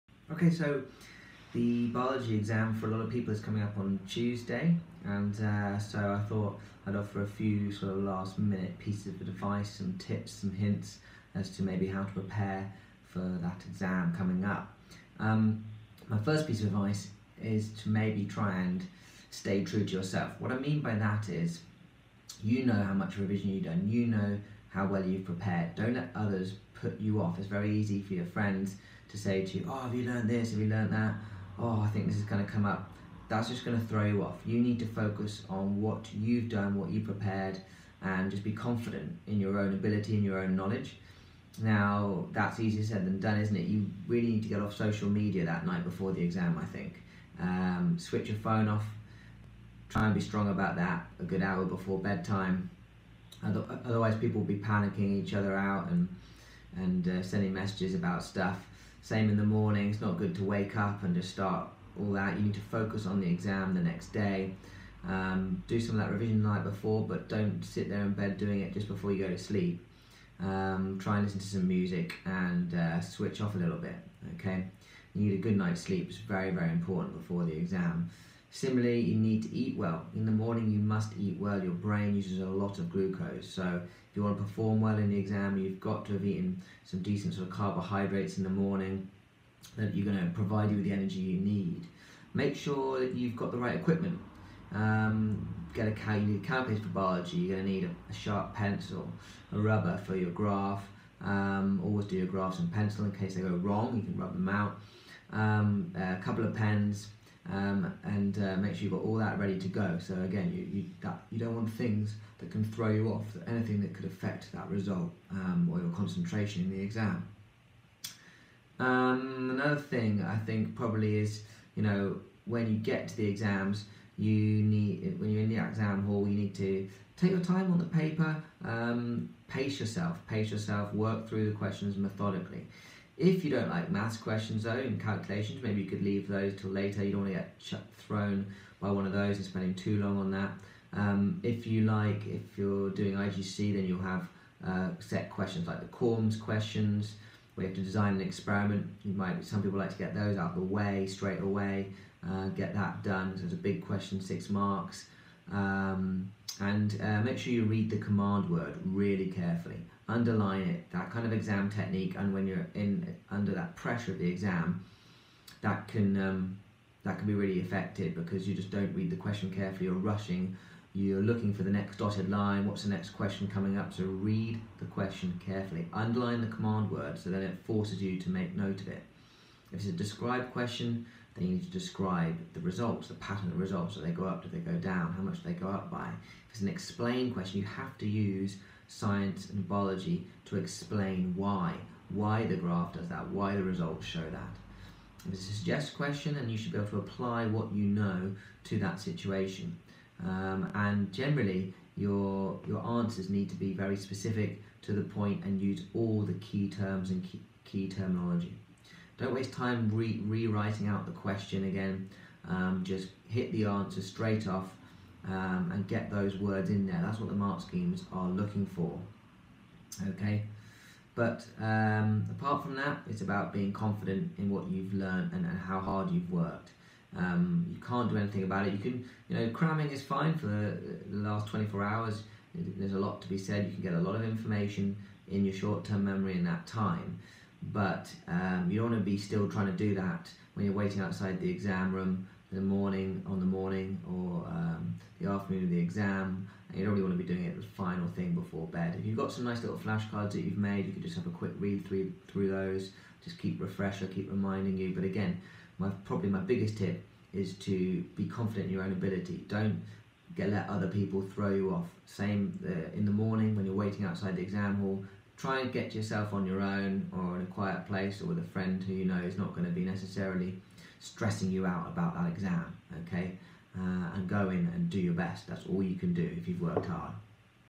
Final words of advice before you sit your GCSE Biology exam! 😰🙏✏️
My students have their Biology IGCSE exam in 36 hours time. So I thought I would share some words of advice about what to do before and during the exam to try and perform at your best.